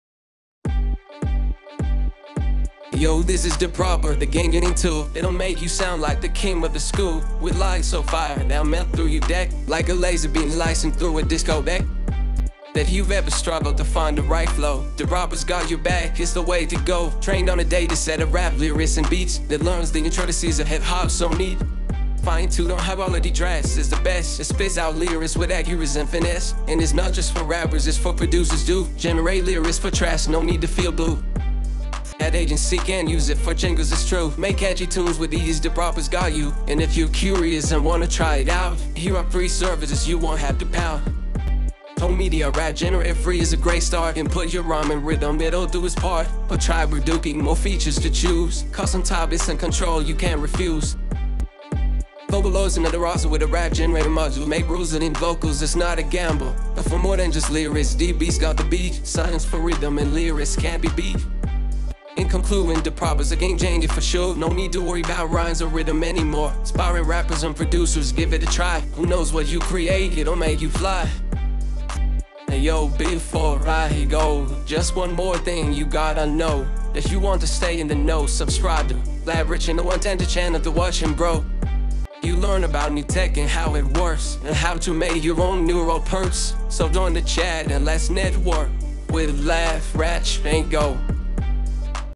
DeepRapper for creating rap with rhyme and rhythm modeling
Check out this rhythmic rap retelling of an article about DeepRapper, an innovative AI-powered rapping tool with rhyme and rhythm modeling.

The voiceover in this video was created using the free app Uberduck.ai, and the video was created using Voice AI With this powerful app, you can synthesize Russian speech and even create deepfake videos from images!